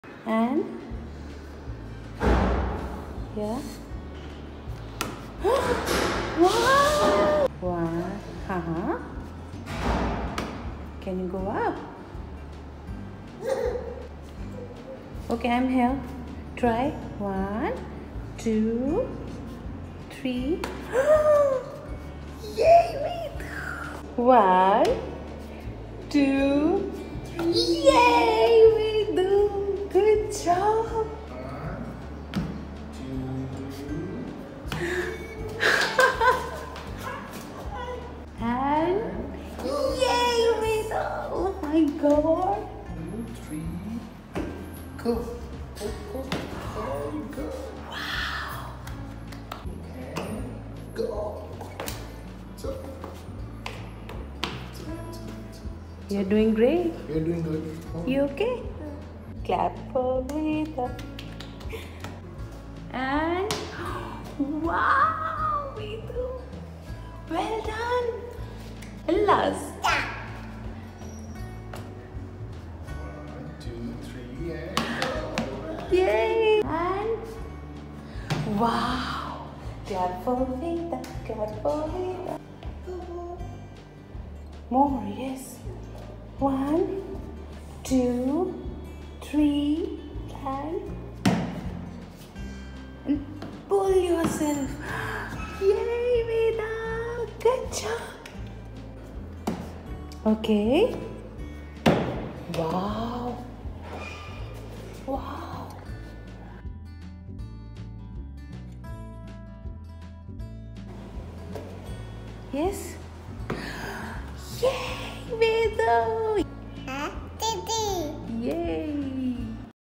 Climbing Stairs Unassisted after months of practice | Down Syndrome India
After months of step up practice on 2 and 4 inch steps/stairs, she finally climbed stairs unassisted today.

Yes, we have been practising this with support from months. She cried for weeks in park, stairs initially, got hurt but never gave up.
It was tough for her due to low muscle tone and also her quad muscles were not strong enough for weight shifting.

And today we are here. She’s the strongest kid ever💪🏻